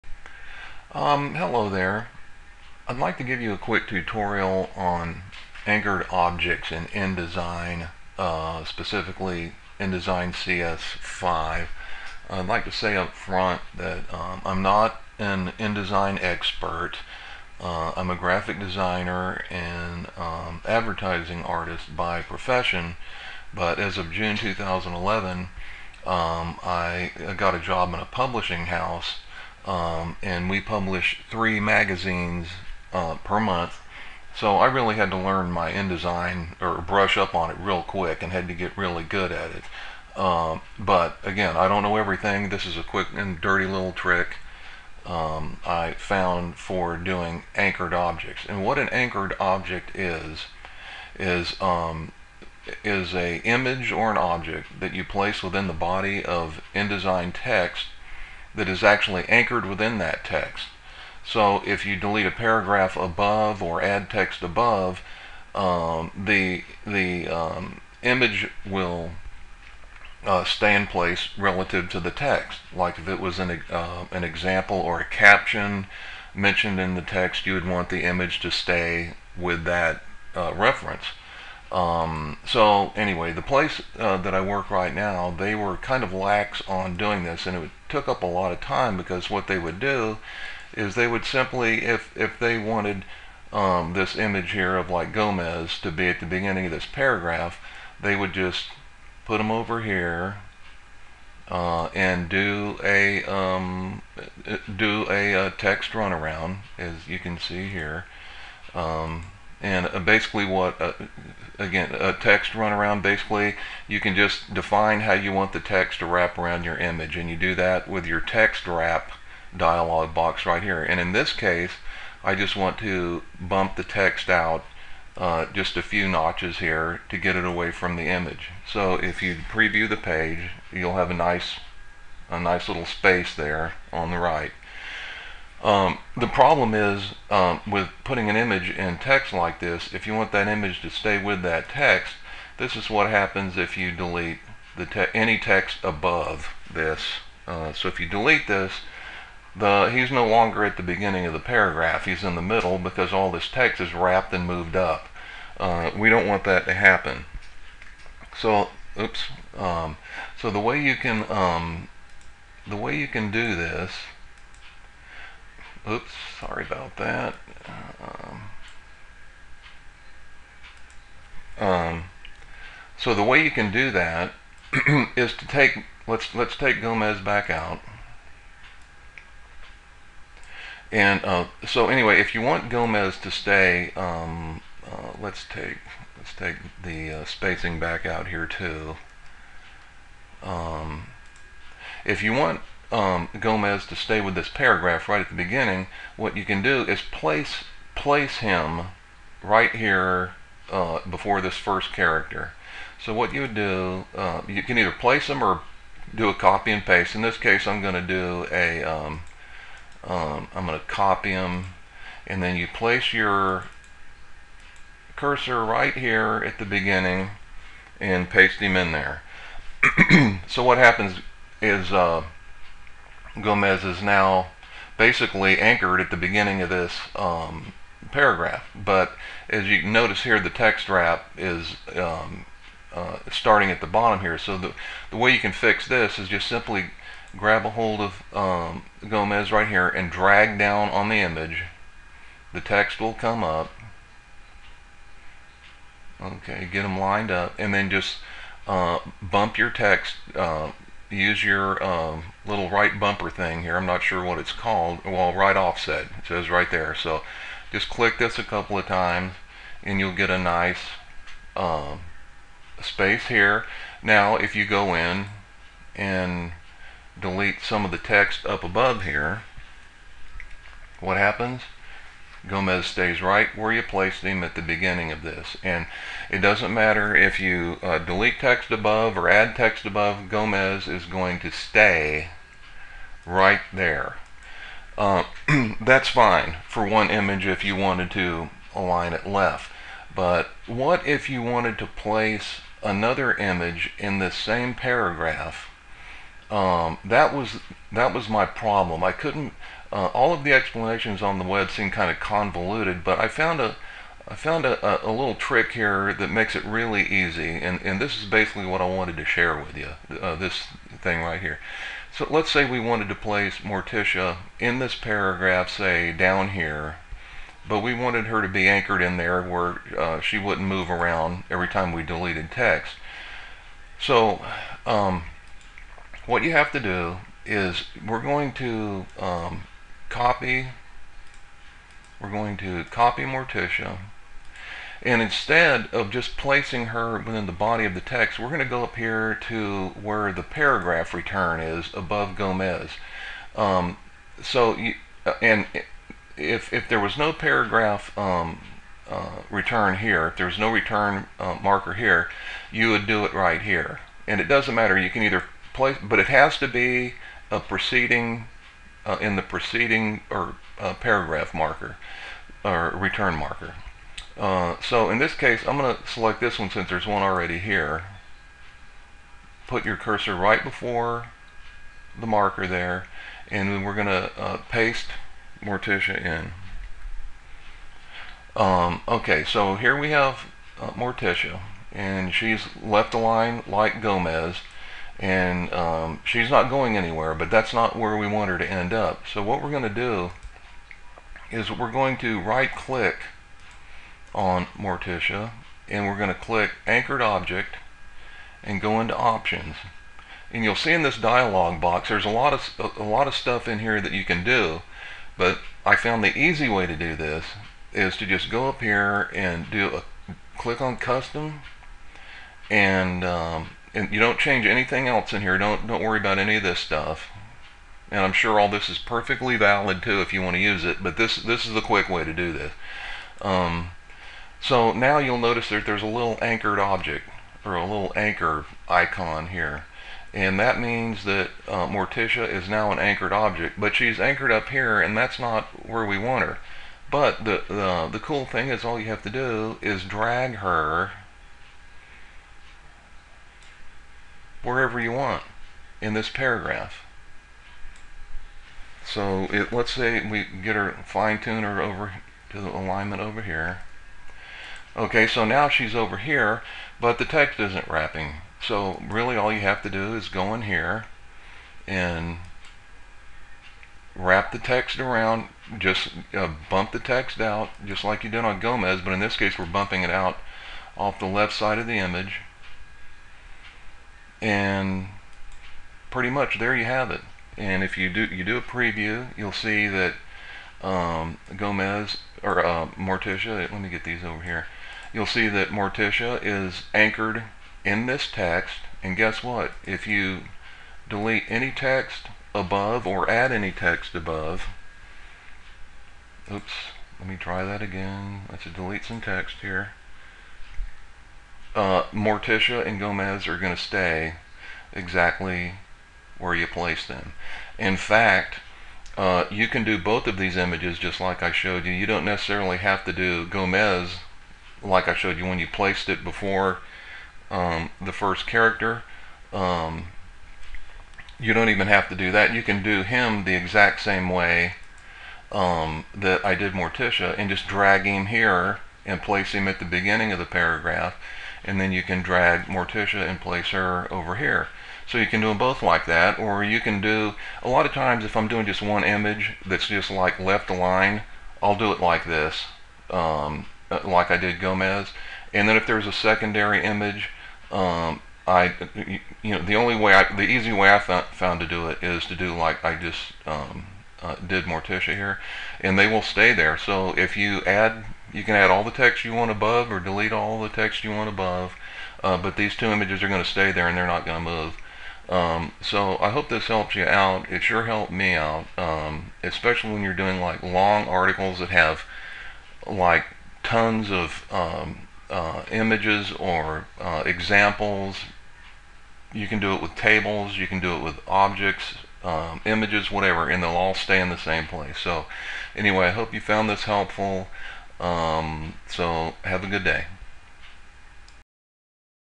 Anchored Objects in InDesign CS5 ...
This is a quick way to place and align your images and objects within InDesign text and have them stay where you put them relative to the text. A little secret I discovered...